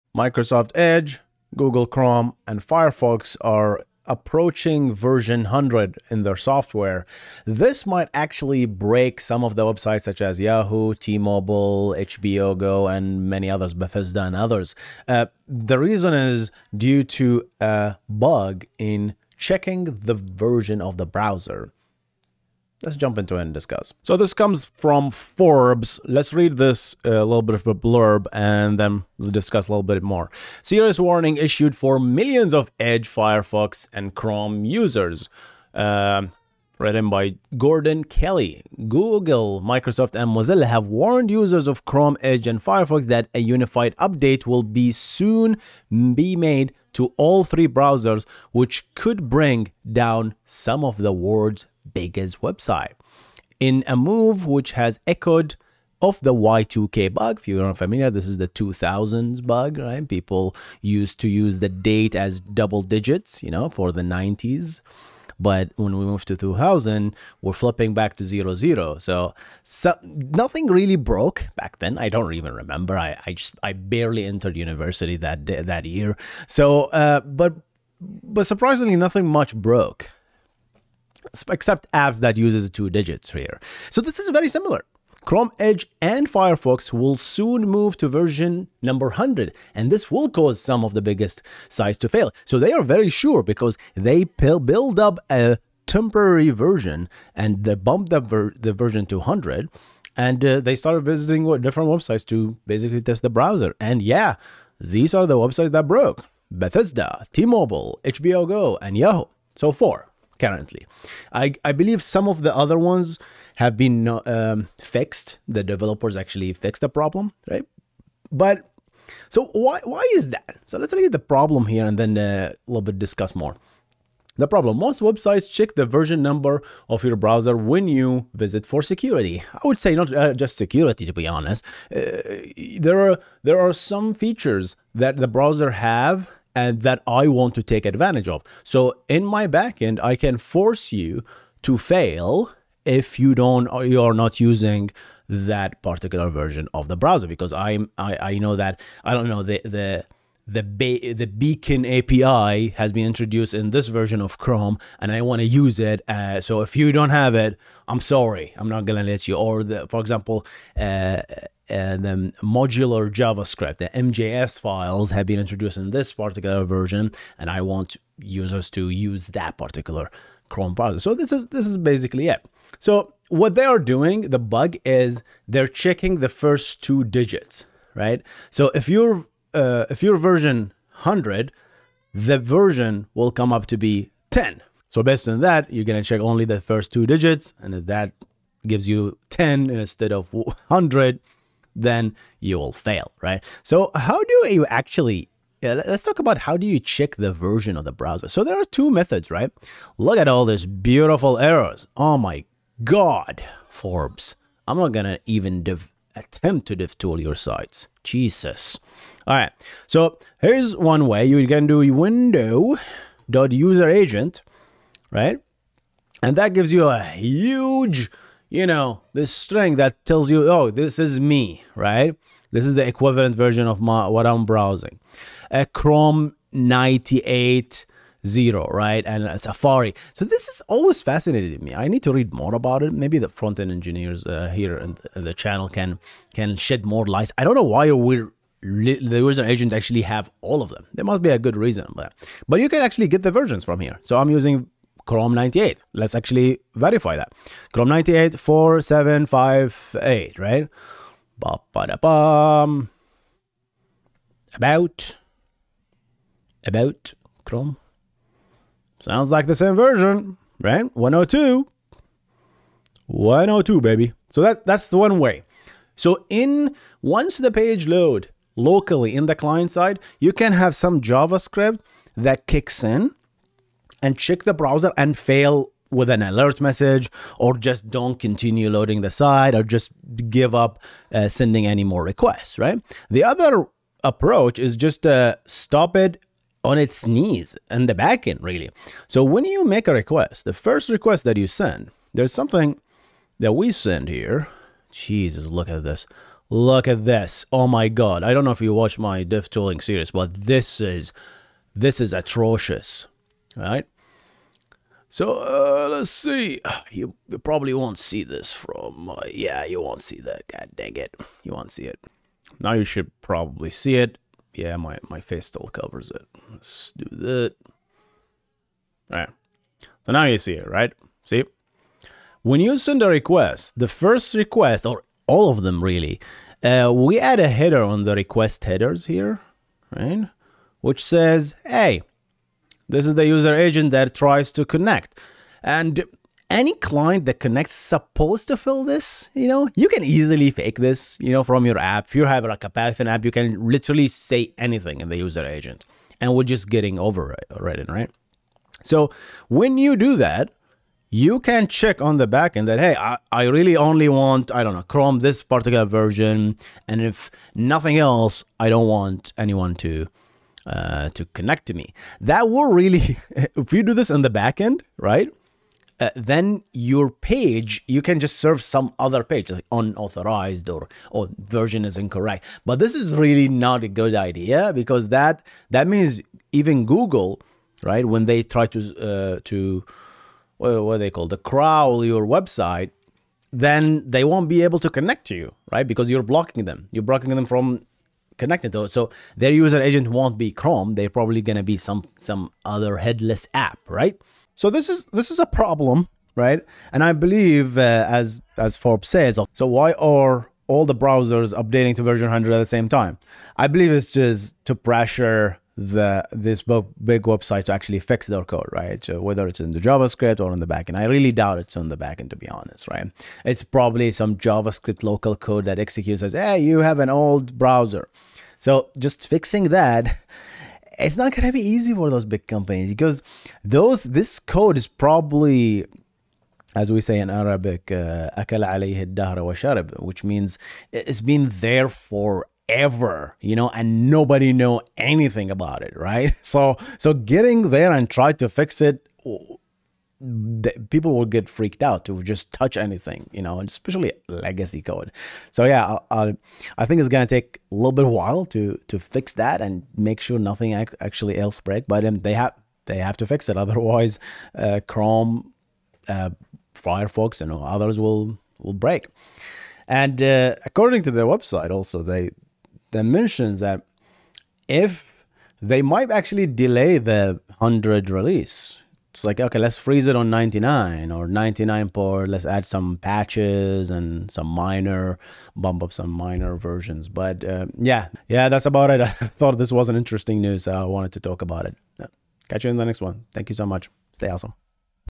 Why the next Chrome version will break websites
Chrome, Edge and Firefox are approaching version 100. that version is 3 digit version where most websites only checked two digits. You can imagine what can go wrong. Lets discuss

Stay Awesome,
Hussein